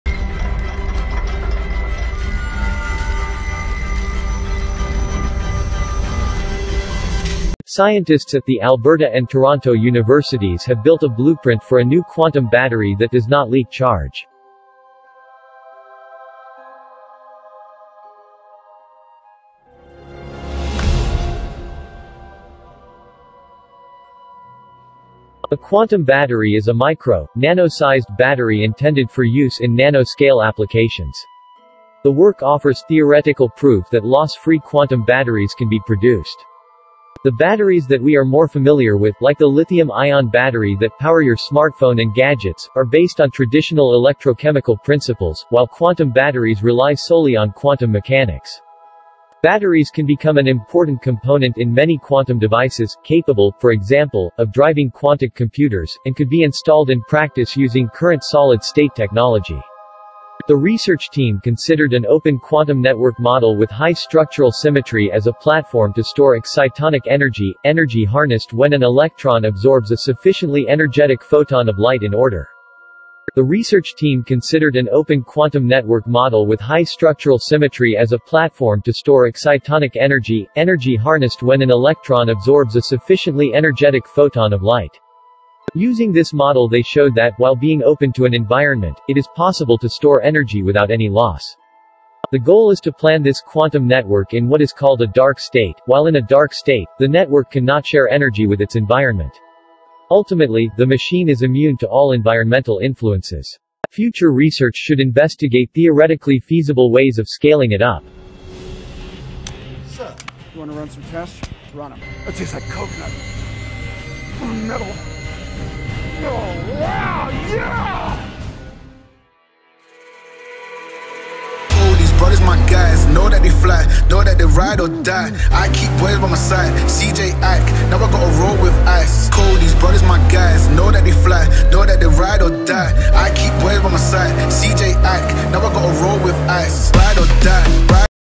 Quantum battery | infinite energy
they created a blueprint for 'quantum battery' that doesn't lose charge